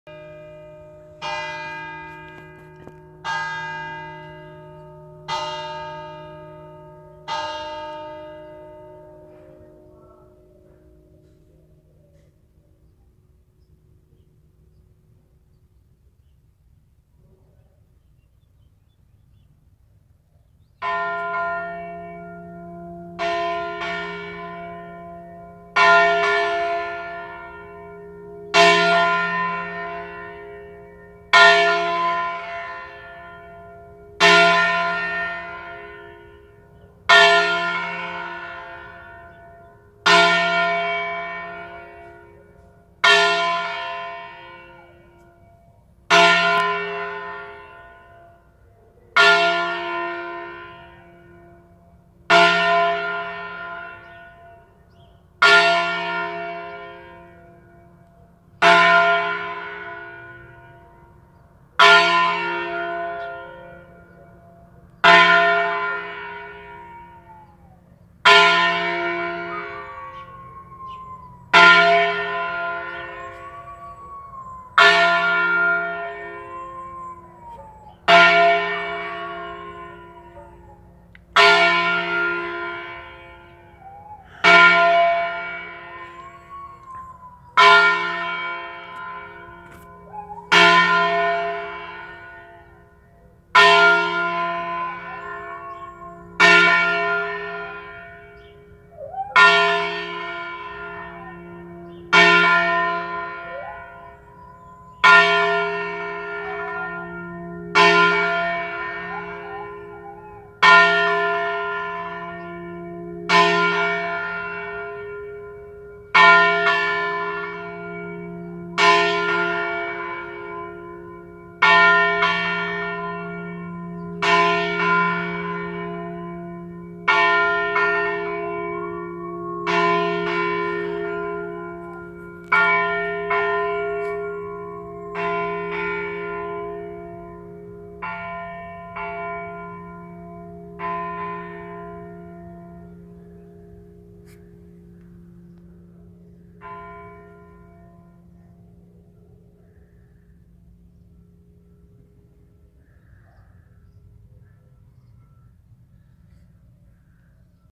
Le campane di Brenno Useria - Chiesa di S. Maria Immacolata
Ricevo e pubblico video delle campane di Brenno Useria, provincia di Varese, con chiamata per la Santa Messa mattutina